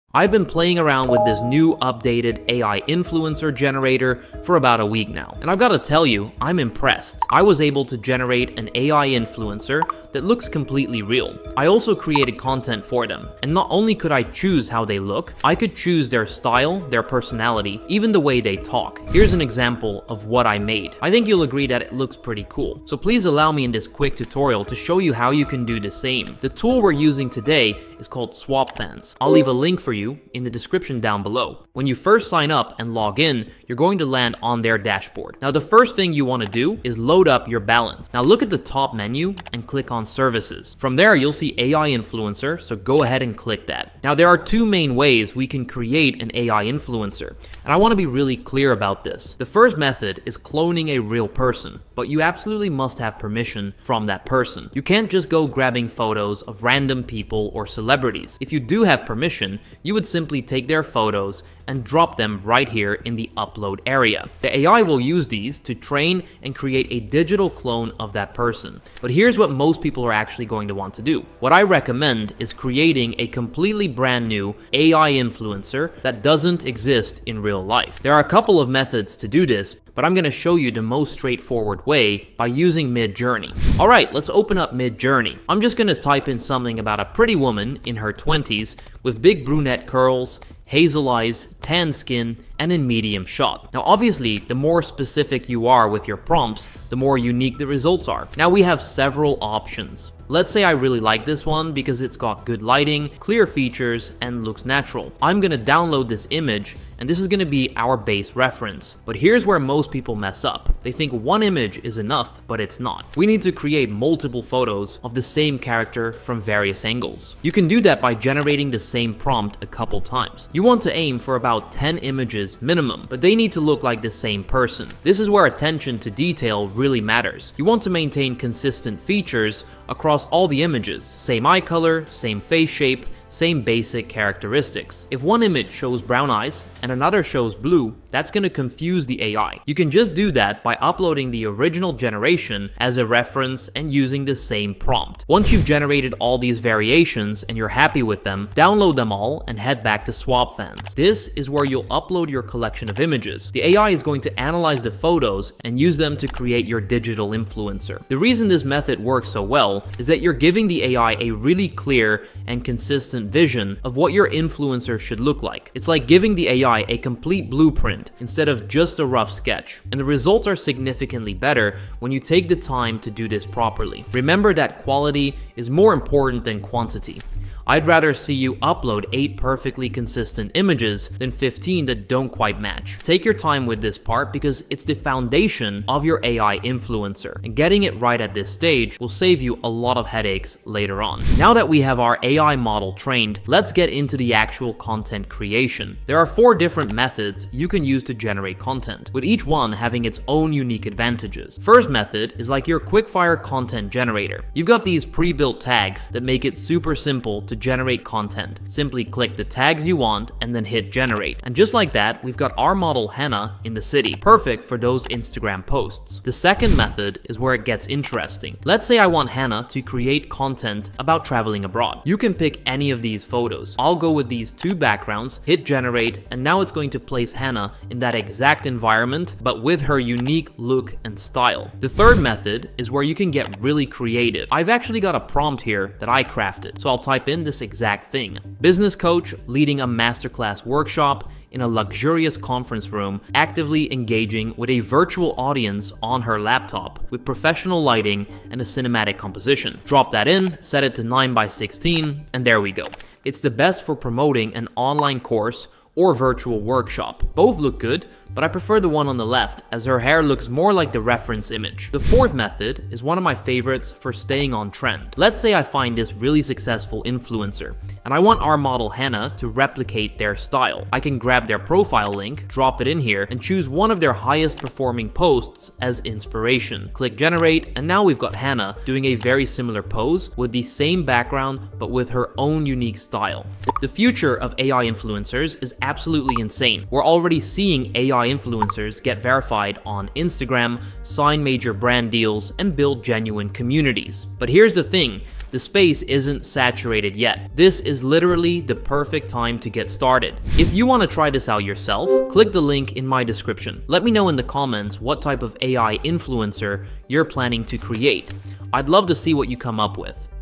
How to Create Consistent Character AI Influencers (Step by Step)
✅ 4 Proven Ways to Make Money with AI Characters

Who needs models in 2025 when you have AI influencer generators that can do it all! In this swapfans tutorial I will show you step by step how you can make your very own AI influencer from one single image using this easy to use AI influencer generator!

I will break it down step by step in this full ai influencer tutorial:

1. How to sign up
2. How to create ai influencer with swapfans
3. How to create pictures including your influencer with ai influencer generator
4. How to customize & edit the generated result

After testing many ai influencer tools, swapfans has got to be one of the, if not the best right now.